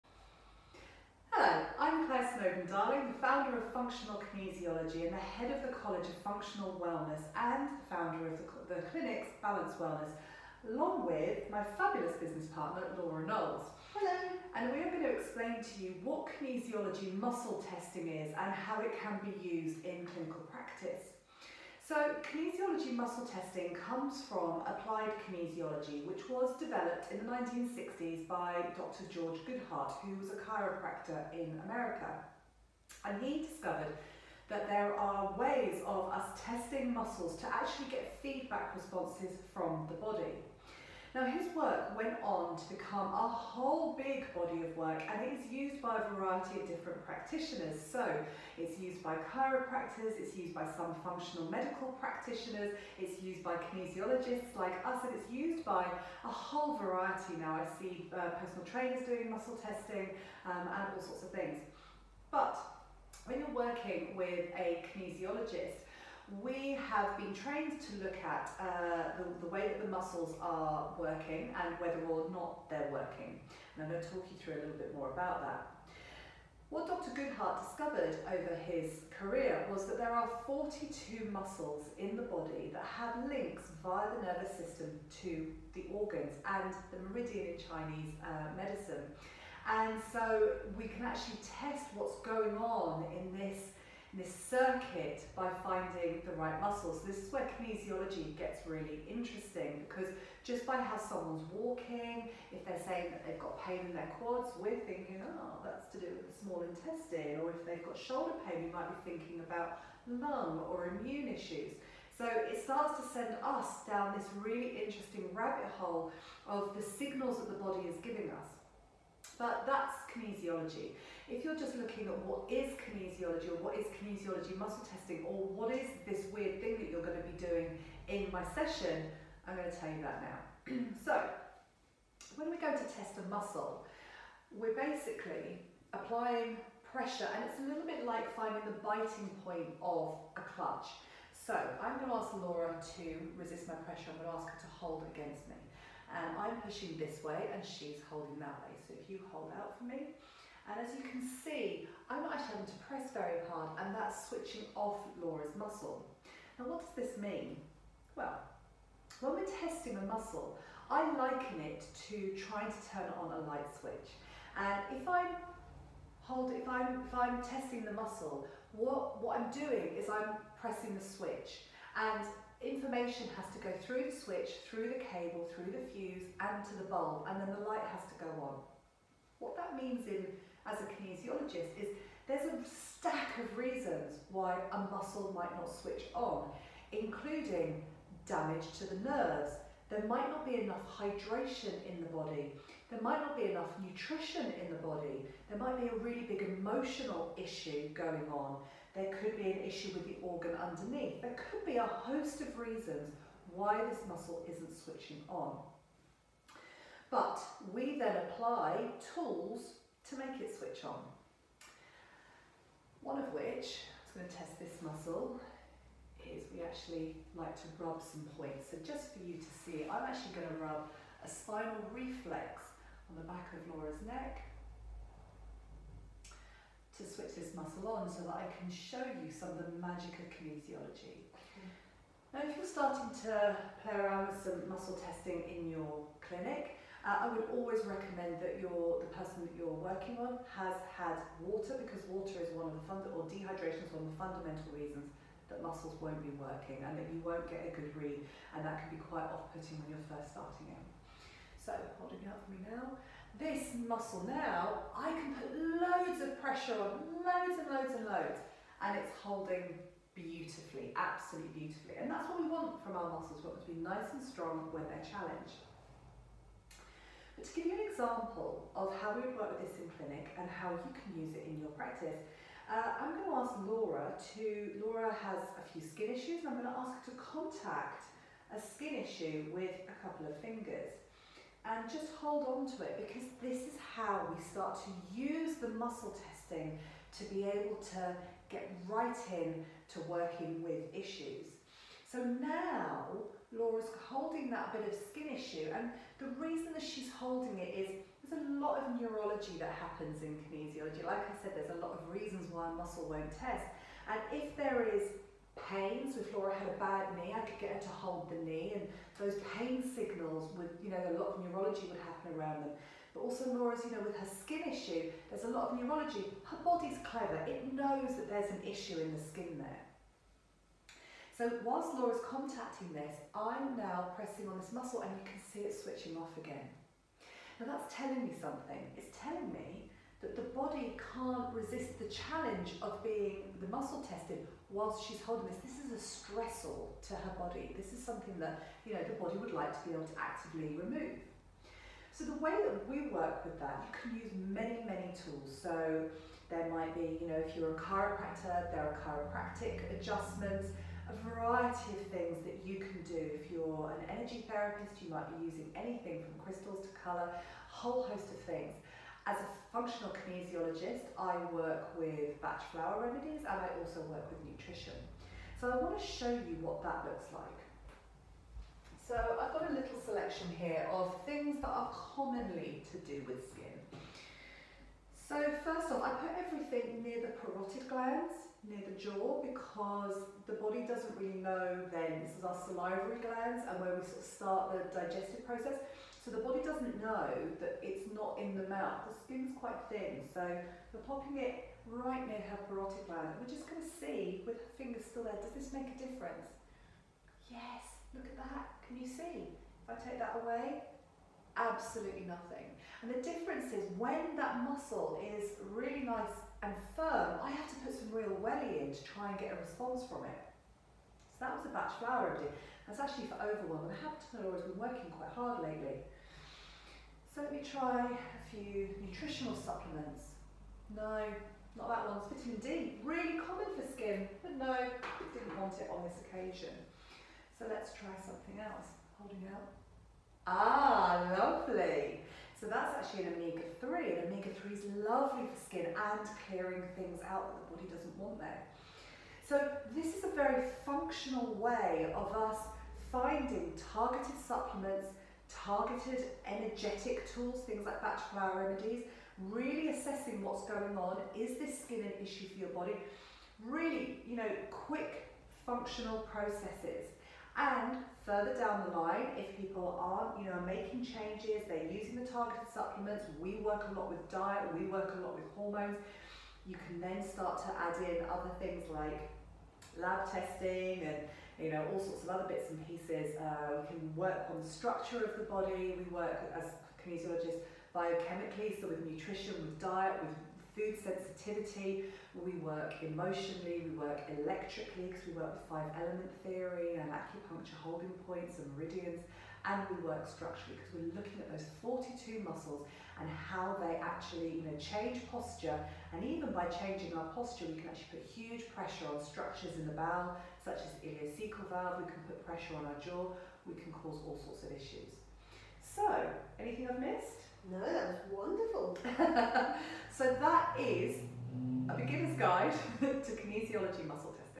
What is Kinesiology Muscle Testing
Claire Snowdon-Darling explains how kinesiology muscle testing works and how we can use it to find where imbalances are in the body and targeted nutrition and tools to correct the imbalances.